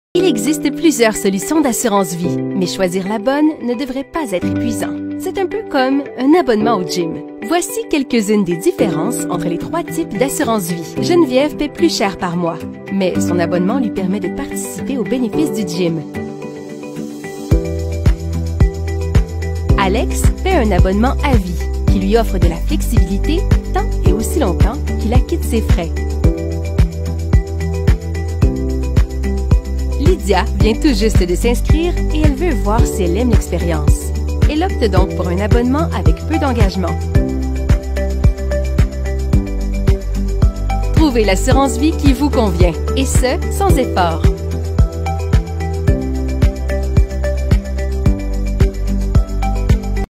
Quelle assurance vie vous convient le mieux ?
Vous avez de la difficulté à vous retrouver parmi les différents types d’assurance vie? Gardez votre énergie, nous sommes là pour vous aider! Parlez avec une représentante ou un représentant Co-operators pour trouver l’assurance vie qui vous convient, et ce, sans effort.